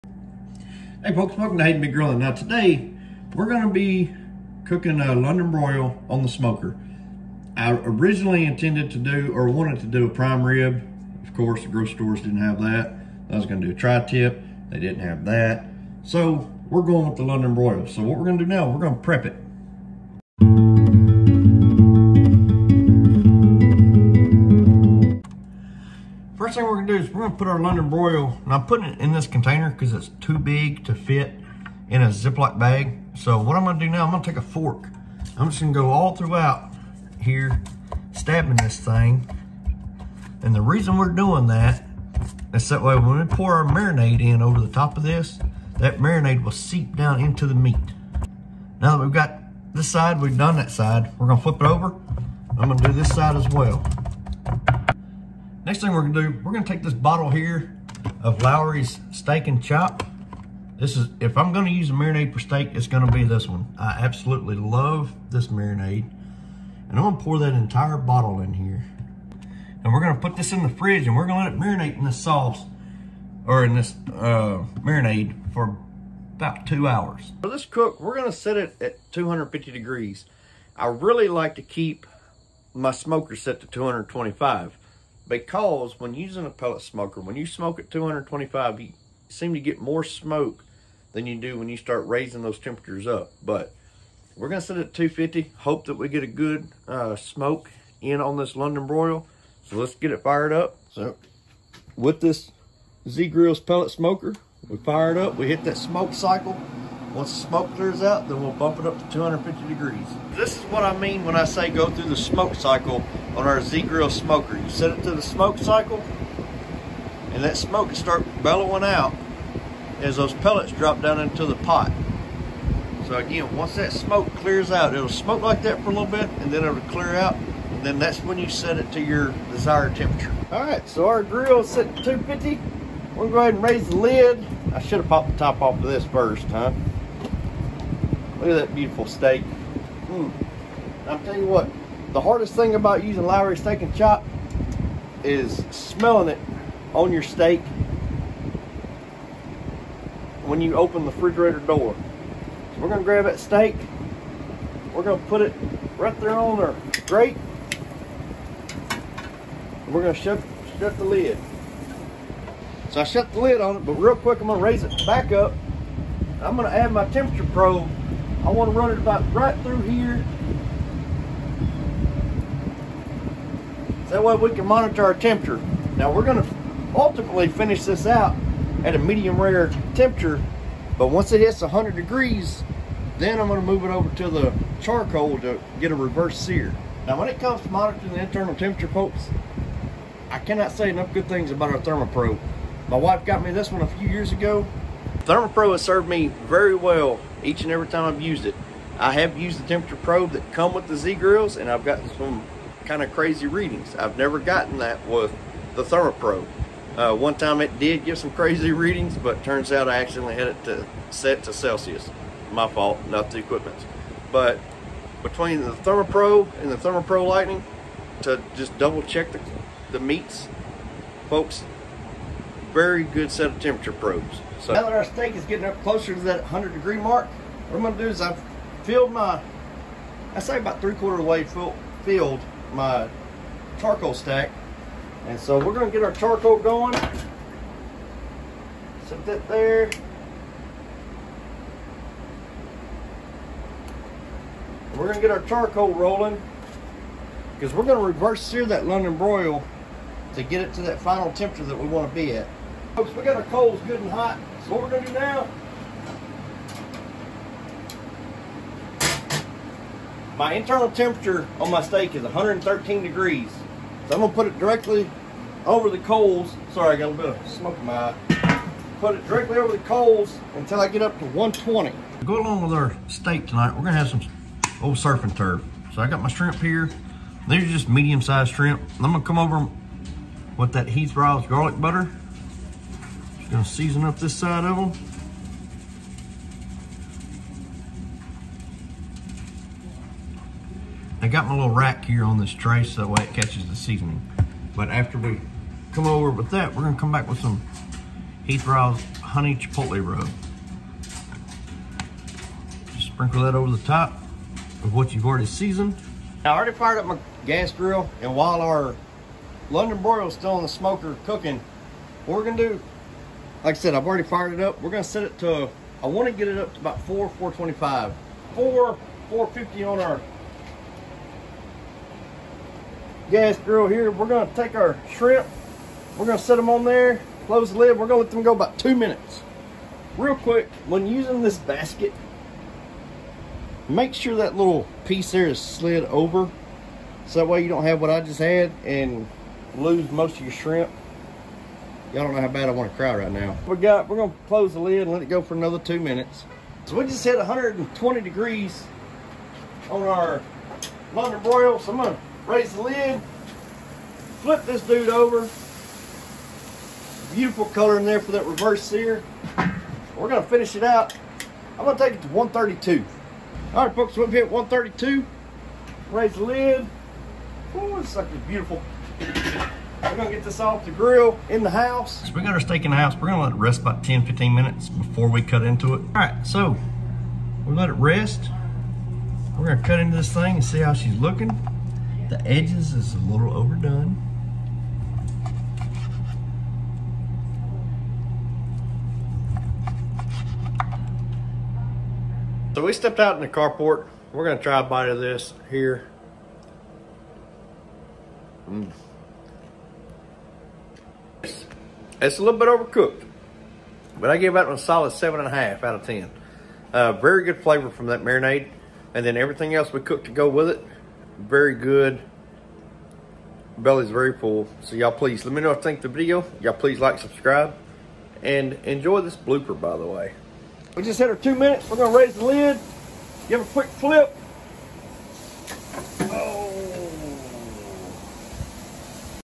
Surf & Turf With a London Broil?
I never woulda thought that a London broil would be sooooo good!!! Give this a try!!

Thermopro

Z-Grills